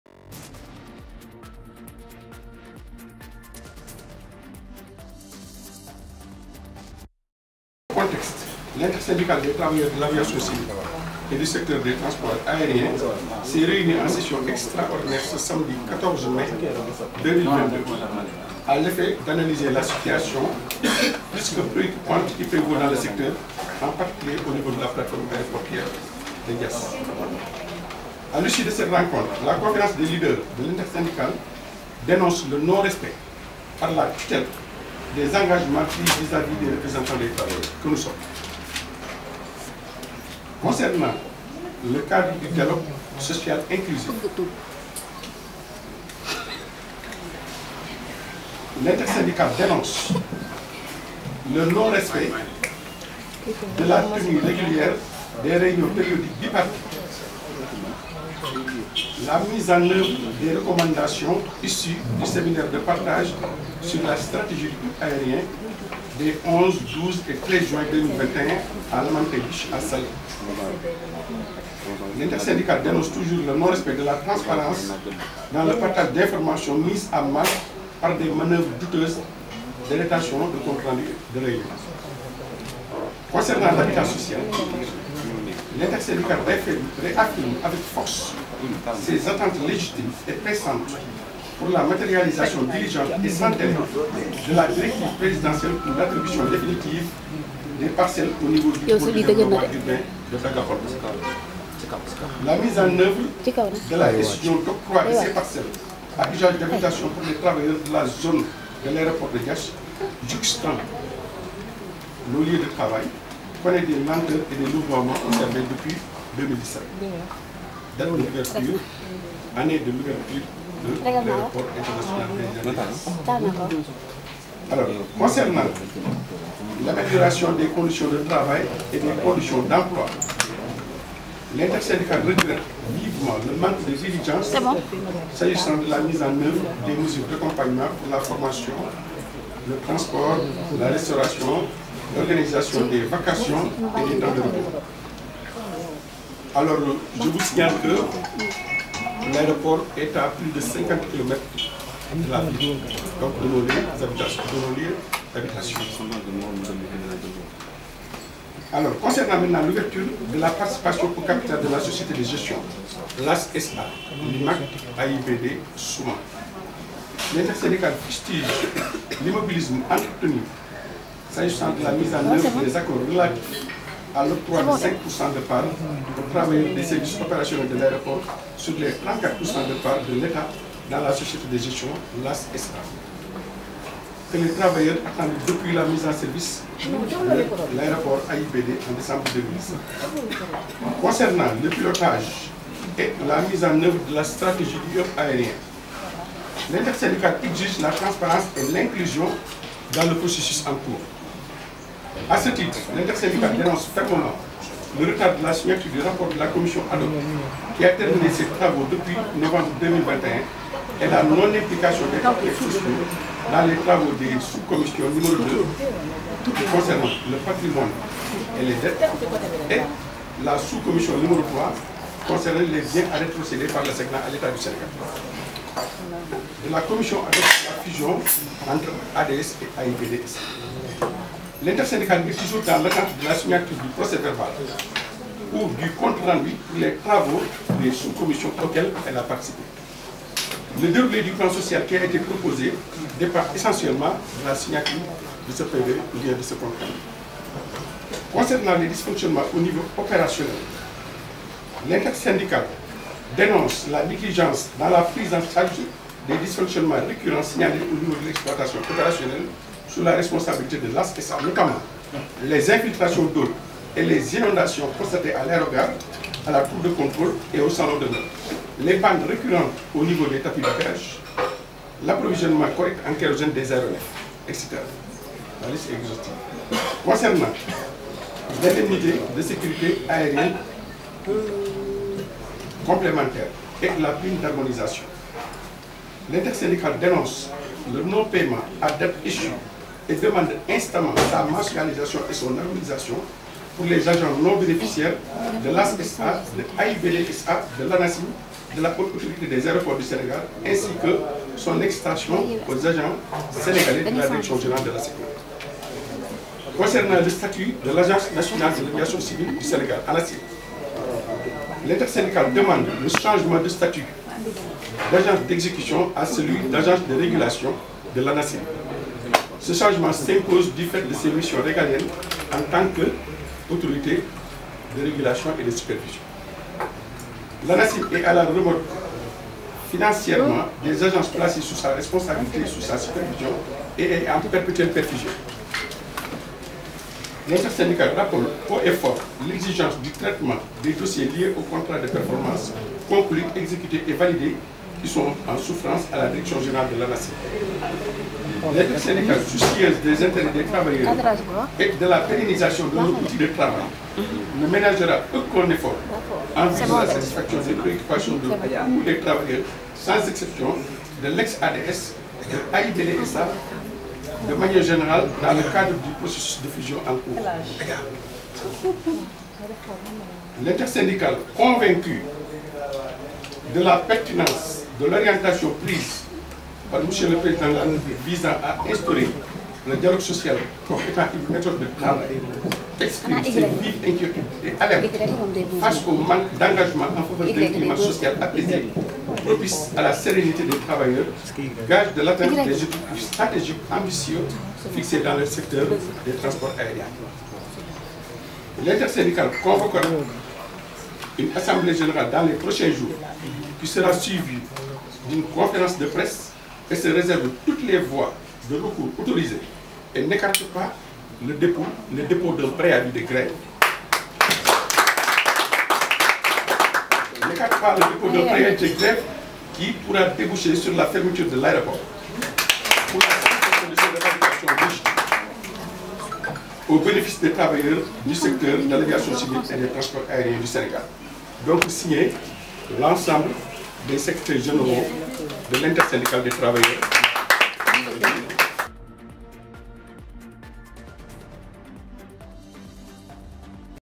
Urgent!l'intersyndicat des travailleurs de l'aéroport alerte l'état et menace de déposer un préavis.
Urgent!l'intersyndicat des travailleurs de l'aéroport alerte l'état et menace de déposer un préavis  de grève